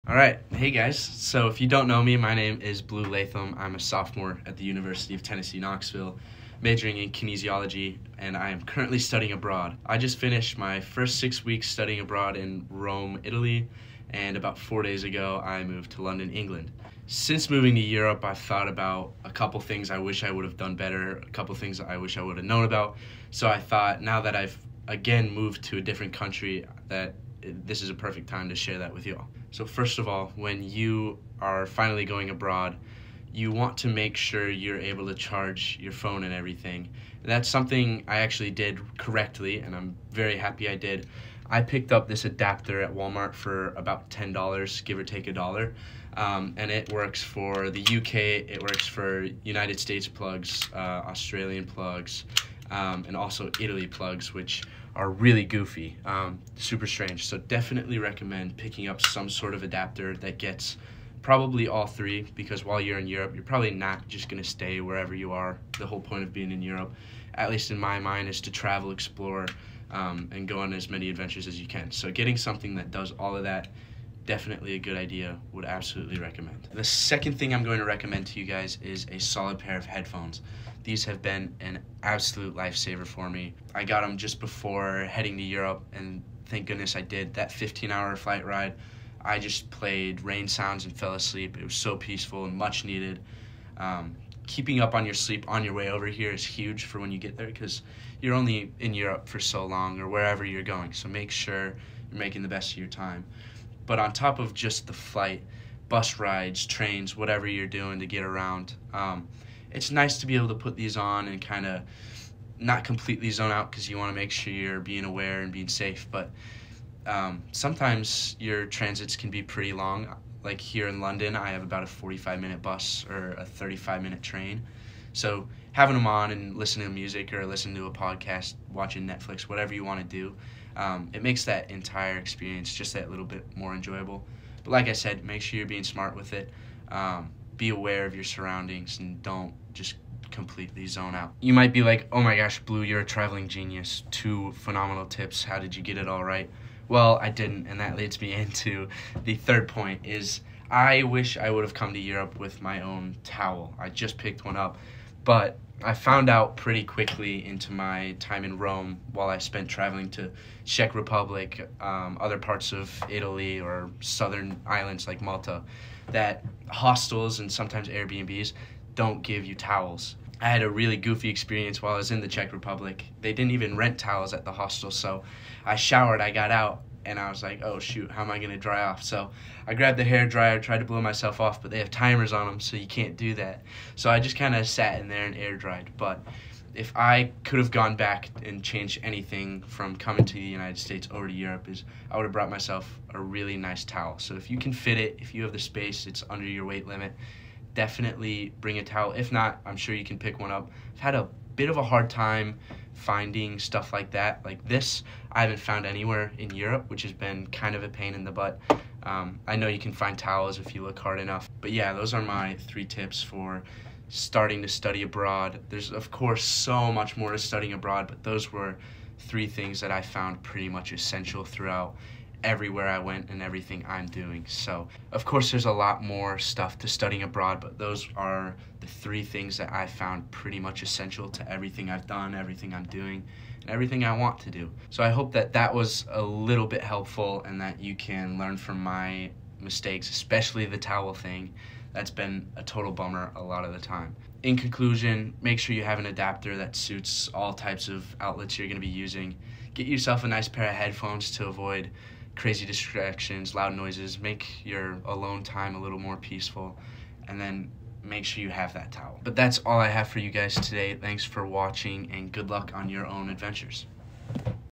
Studying Abroad Must-haves
Here are some of the things I have found most important over my first two months studying abroad, living in two countries and traveling to five. Some of these may be kind of obvious but definitely needed!